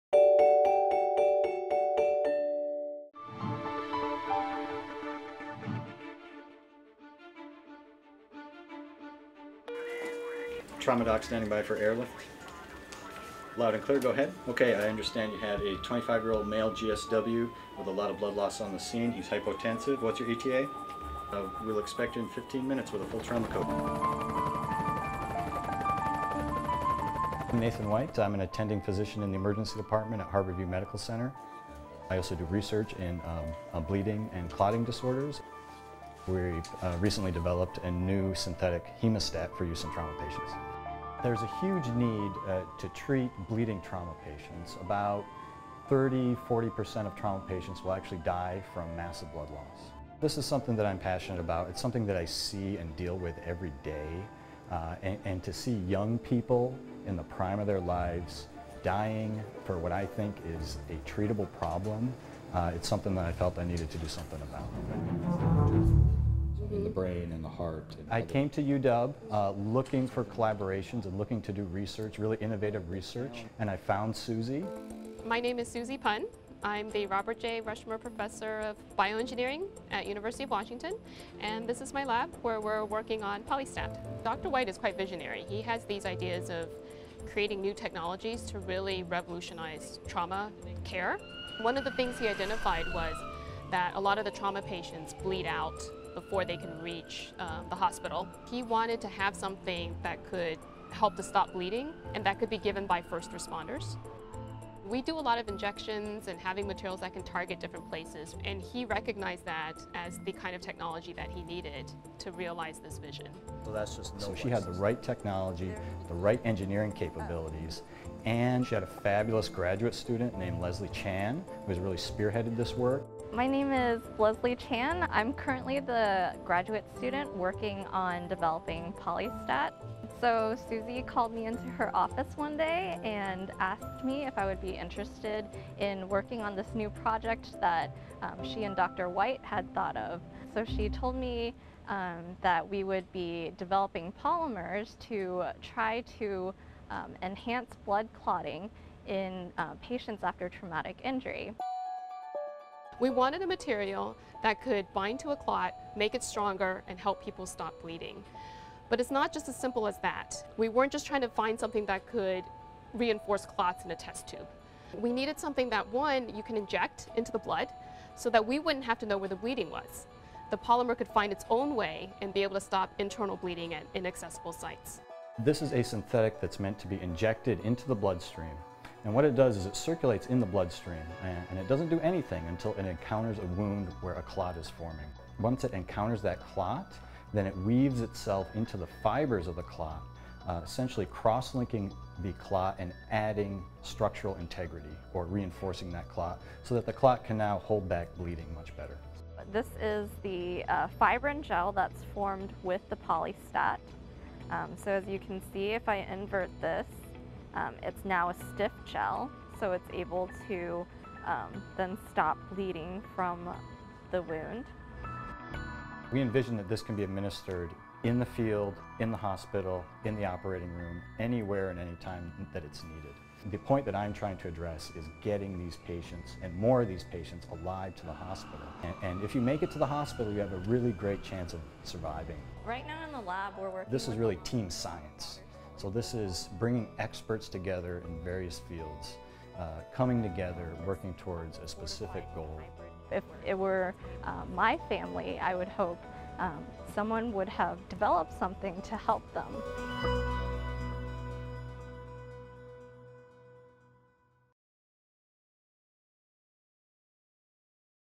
Life-Saving Blood-Clotting Polymer
Meet University of Washington professors and student researchers who are working together to create a blood clotting polymer that will increase the survival rate of trauma victims.

Nathan White, M.D., adjunct assistant professor, Bioengineering and Mechanical Engineering, UW
Suzie Pun, professor, Bioengineering, adjunct professor, Chemical Engineering, UW

04/26/2015